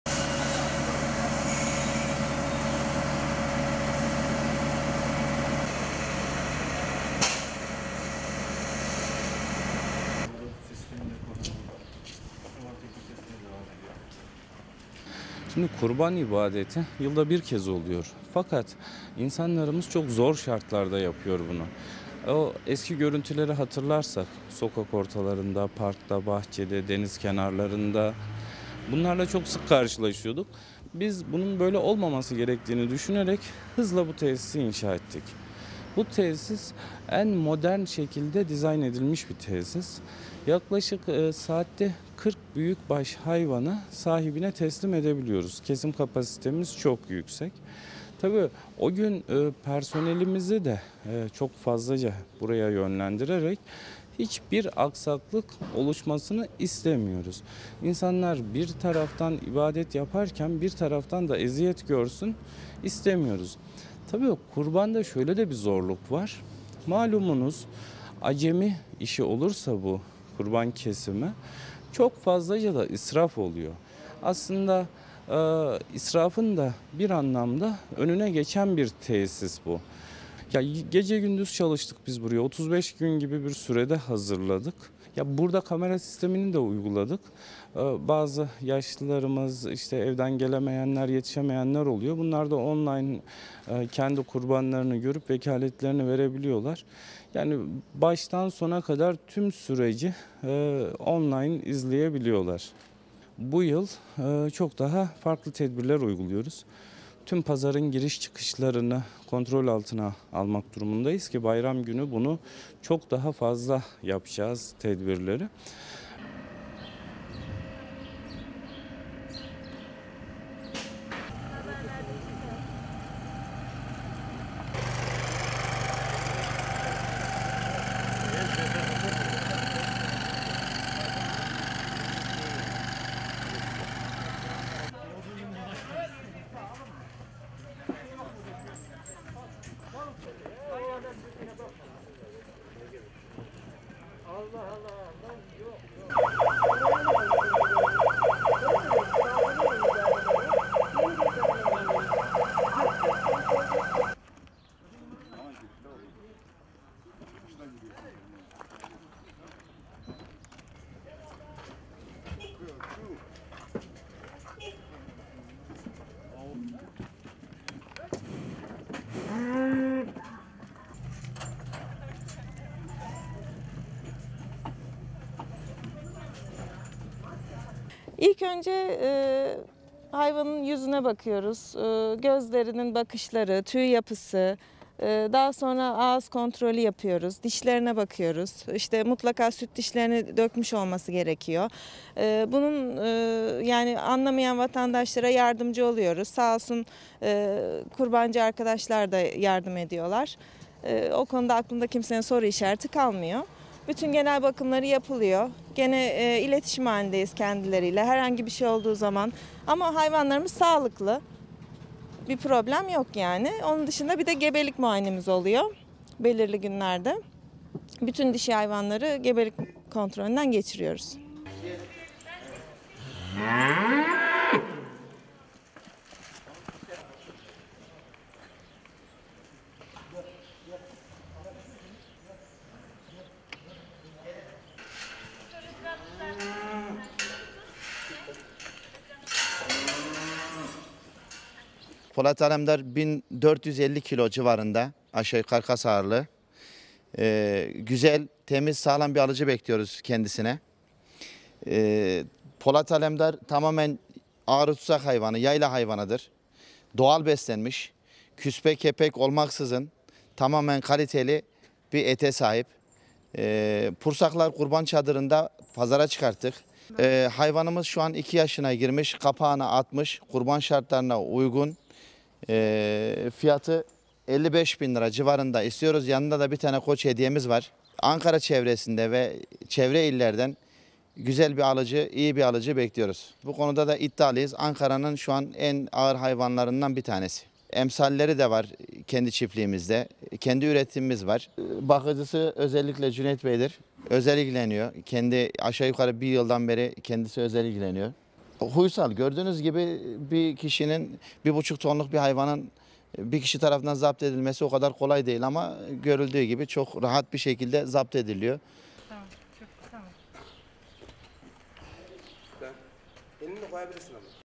Bir Buçuk Tonluk 'Polat Alemdar' Kurban Pazarında Araba Fiyatına Alıcı Bekliyor
Ankara’nın Pursaklar ilçesinde satışa çıkarılan bin 450 kg ağırlığındaki Polat Alemdar, alıcısını bekliyor. Kurban pazarının en gözde kurbanlığı olan Alemdar, 55 bin TL ‘den satışa çıkarılırken satıcı; dev danayı satın alacak kişiye koç hediye edecek.

Pursaklar Belediyesi Kurban Pazarında satışa çıkarılan bin 450 kilogram ağırlığındaki Polat Alemdar, 55 bin TL’den alıcını bekliyor. Kendisi için özel bir alan ayrılan dev kurbanlığı alanlara ise koç hediye edilecek. 2 yaşındaki simental cinsi Alemdar özel olarak yetiştirildi.
Pursaklar Belediyesi, Kurban Bayramı için modern bir tesis kurdu. Kısa süre içinde hazırlanan tesiste Korona virüs nedeniyle güvenlik önlemleri en üst düzeye çıkarıldı. Kurban pazarına giriş yapan vatandaşların öncelikle ateş ölçümleri yapılıyor, ardından dezenfektan kullanmaları isteniyor, talep halinde maske ve eldiven veriliyor.
Belediye ekipleri tarafından sabah ve akşam olmak üzere çadırlarda ve tesis içinde genel dezenfeksiyon işlemleri yapılıyor. Kurban ibadetini yapmak isteyen vatandaşlar, önce vekaletini verecek sonrasında kendisi için hazırlanan alanda kurbanlıkların kesim işlemlerini ve hazırlanışını izleyebilecek. Alana gelemeyen vatandaşlar da online seçeneği kullanarak vekalet verebilecek ve yine kurbanlıklarının kesim ve hazırlanışının online takibini yapabilecek. Ayrıca kurban pazarında bulunan veteriner hekimler de rutin olarak kurbanlıkları muayene ederek sağlık durumlarını gözlemliyor. Vatandaşların talepleri halinde sorularını yanıtlıyor.
" İsrafın da bir anlamda önüne geçen bir tesis bu"
Pursaklar Belediye Başkanı Ertuğrul Çetin vatandaşların Kurban ibadetini sorunsuz gerçekleştirebilmesi için gerekli tedbirlerin alındığını belirterek," Kurban ibadeti yılda bir kere oluyor. Eski görüntüleri hatırlarsak parklarda, bahçelerde kesimler yapılıyordu. Biz bunun böyle olmaması gerektiğini düşünerek bu tesisi inşa ettik. Bu tesis en modern şekilde dizayn edilmiş bir tesis. Bir saatte yaklaşık 40 büyükbaş hayvanı sahibine teslim edebiliyoruz. Personel sayımızı da artırarak hiçbir aksaklık olması istemiyoruz. İnsanlar bir taraftan ibadet yaparken bir taraftan zorluk yaşasın istemiyoruz. Kurban da şöyle bir zorluk var. Kurban kesimi acemiler tarafından yapılırsa çok fazla israf oluyor. İsrafın da bir anlamda önüne geçen bir tesis bu. 35 gün bir sürede hazırlandı . Burada kamera sistemini devreye koyduk. Yaşlılarımız için yetişemeyenler için online sistem kurduk. Vekaletlerini buradan verebiliyorlar. Batan sona tüm süreci izleyebiliyorlar. Bu yıl çok daha farklı tedbirler uyguluyoruz. Bayram günü daha artacak tedbirlerimiz. Giriş çıkışlarda özellikle" dedi
" Ankara'nın en ağır hayvanlarından biri"
Bin 450 kiloluk dana için özel bakım uygulandığını kaydeden Nurullah Ceyhan ise “ Polat Alemdar bin 450 kilo civarında alıcı bekliyoruz kendisine. Yayla hayvanıdır. Doğal beslendi. Küspe ve kepek olmaksızın tamamen kaliteli bir ete sahip Pursaklar Belediyesi Kurban Çadırında satışa çıkacaktır. Hayvanımız şuan 2 yaşına girmiş. Kapağını atmış. Kurban olmaya uygundur. Fiyatı 55 bin istiyoruz. Yanında koç hediyemiz var. Ankara ve çevre illerden alıcılarını bekliyoruz. Bu konuda iddialıyız. Ankara'nın en ağır hayvanlarından biri. Emsalleri de var kendi çiftliğimizde. Bakıcısı özel ilgileniyor. Hayvan güzel bir hayvan. Uysal bir hayvan bir kişinin 1 buçuk hayvanı baş edebilmesi kolay değil ama ördüğünüz gibi kolaylıkla kontrol ediliyor” diye konuştu.